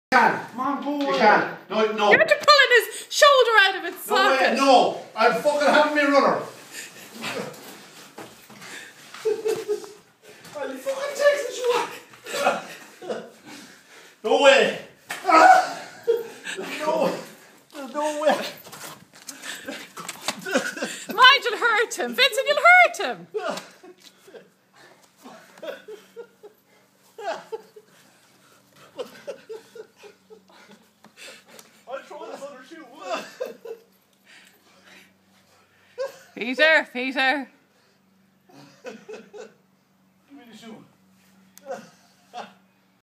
Give me back my trainers! Vincent and Peter - Kilkenny, Ireland
Dad - I am going for a cycle...with your trainers (runners).

Oh no you are not .I need them for my walk!

If you want them - come and take them !